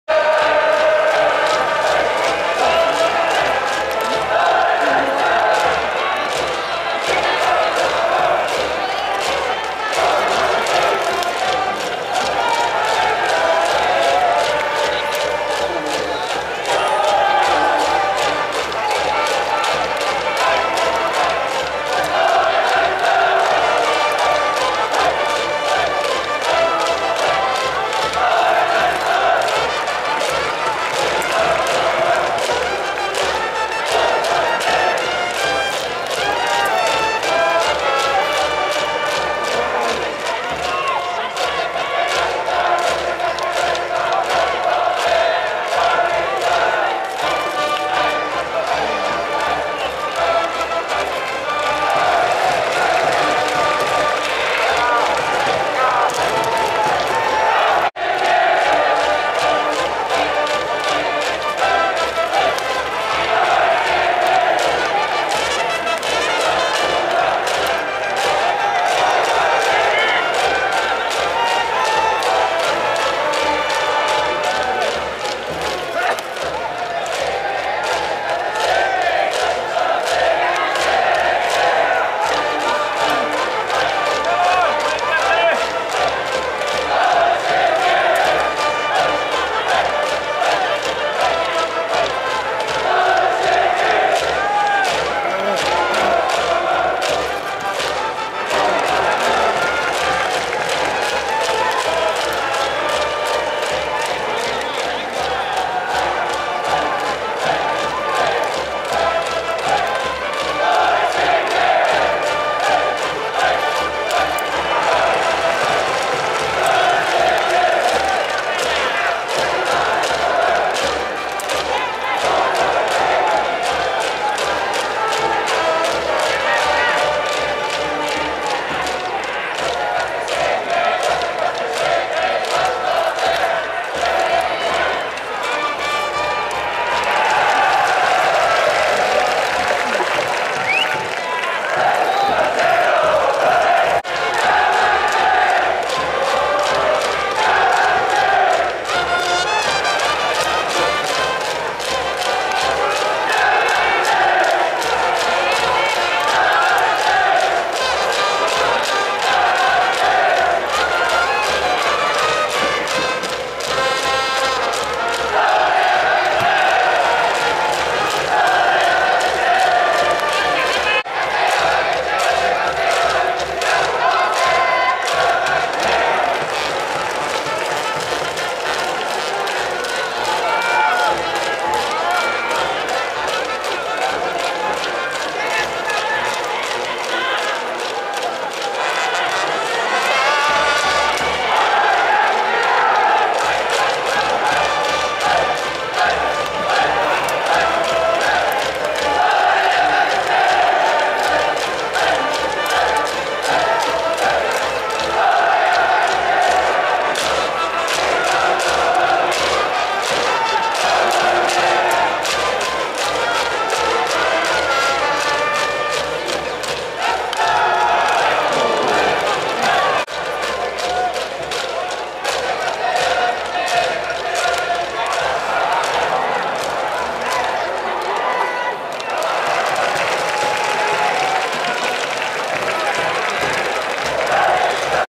2016/6/11 山下仁 （上武大学） 4年春 141km/h 計測
山下 仁　（兵庫・須磨翔風高校⇒上武大学）　180cm80kg　右右　4年生

141km/h